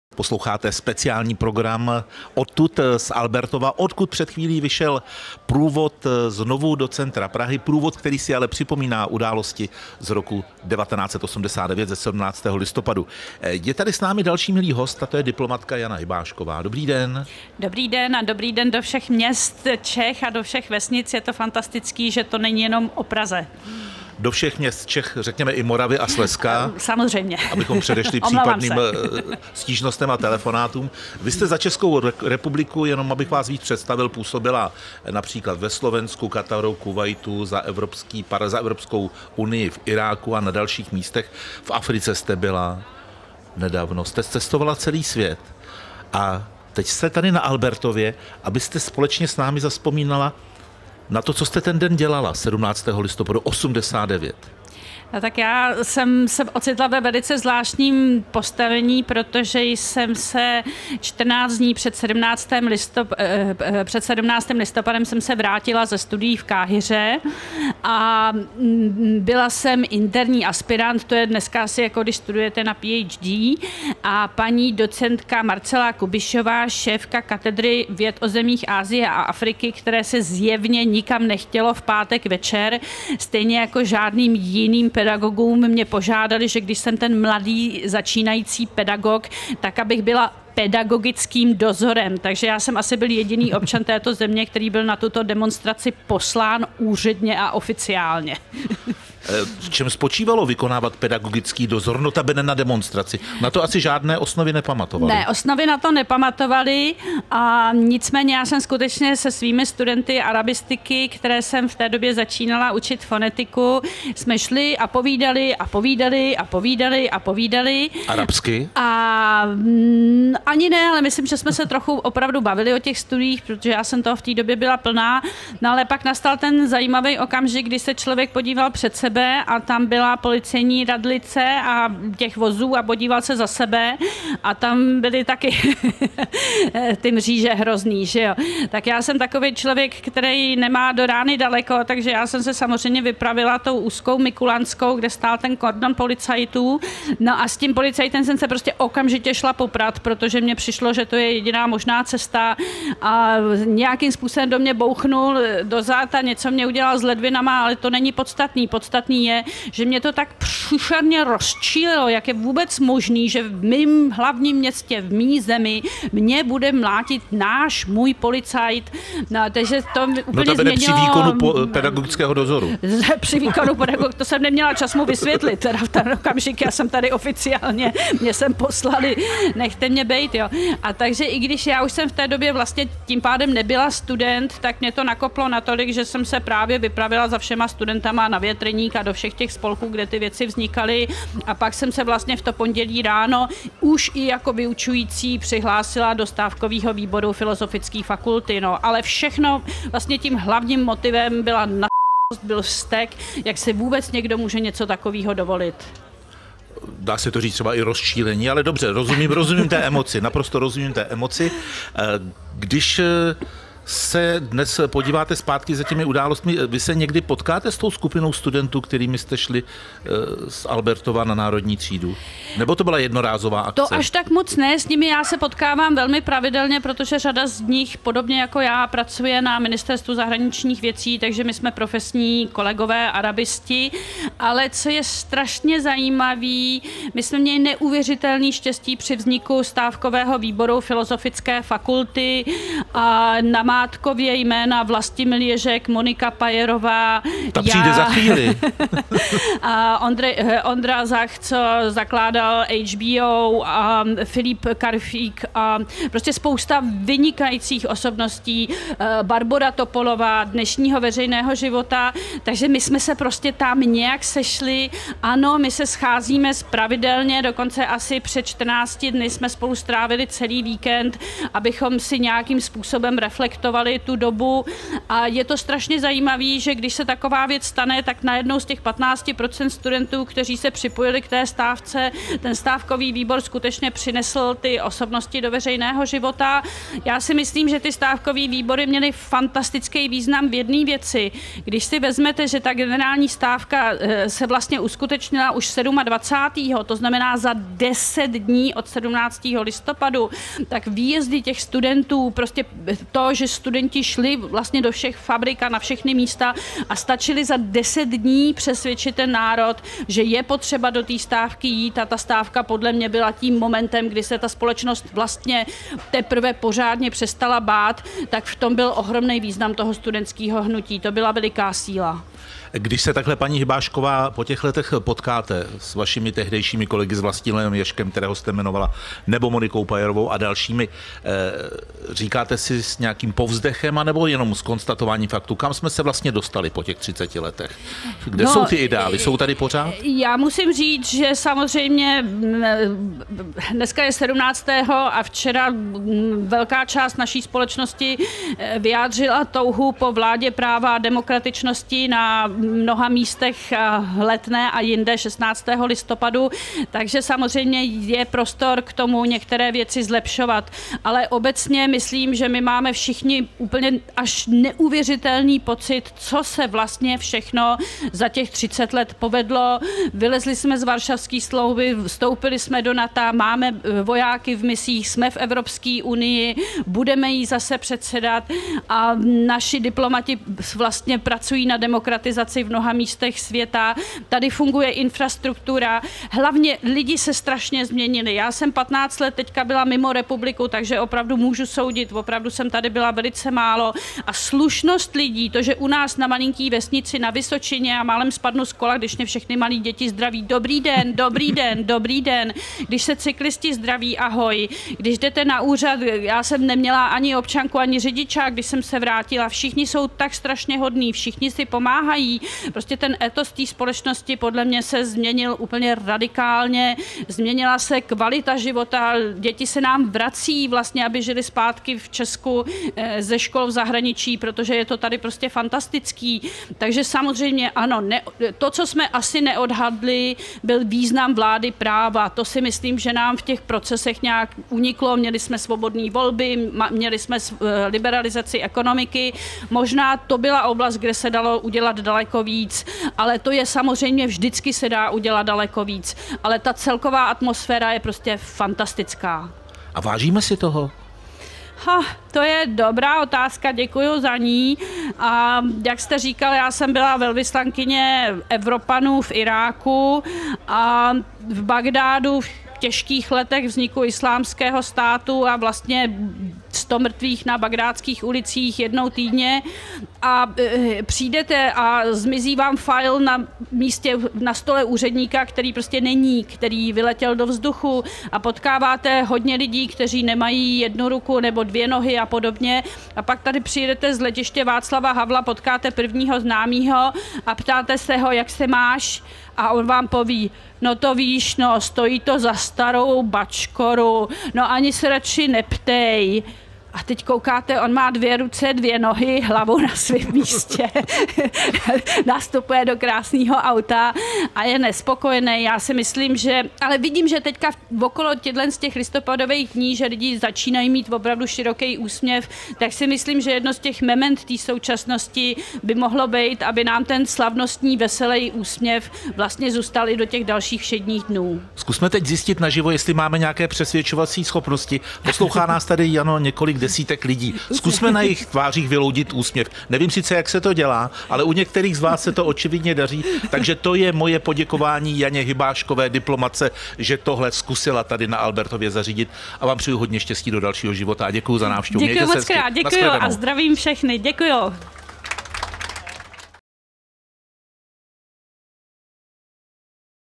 Jana Hybášková: Étos společnosti se změnil, zvýšila se kvalita života
V roce 1989 se osobně účastnila demonstrací, v novém režimu působila jako velvyslankyně v mnoha zemích. „Stávkové výbory přinesly zajímavé osobnosti do veřejného života,“ vzpomíná. „Studenti zvládli za 10 dnů přesvědčit národ, že má do stávky jít, to byla ohromná síla.“ Za posledních 30 let se toho prý také povedlo hodně. „Lidé se strašně změnili, jde třeba o slušnost lidí. Když jdete na úřad, jsou na vás všichni hodní a pomáhají si. Celková atmosféra je fantastická.“